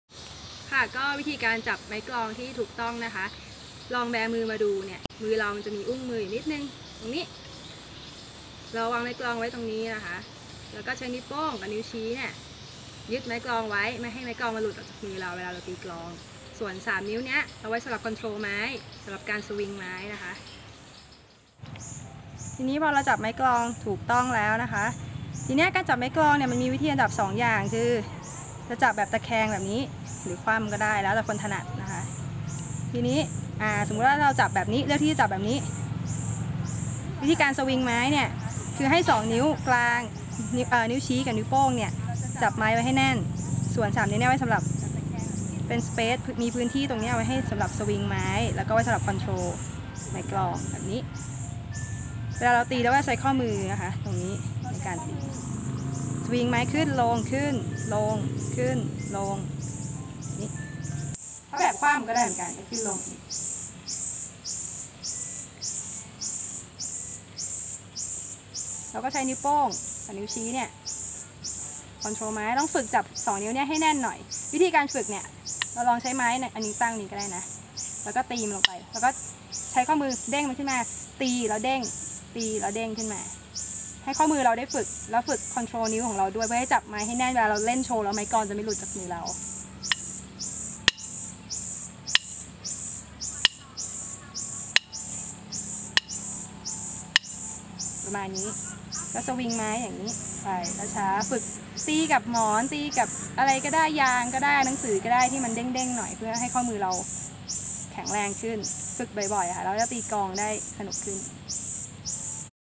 Untitled Background - การจับไม้กลอง
วีดีโอนี้ ทำขึ้นมาเพื่อหวังเพื่อจะเป็นประโยชน์ให้น้องๆ หรือใครก็ตามที่อยากจะเริ่มตีกลองนะคะ แล้วจะทำคลิปต่อๆไปเรือยๆค่ะ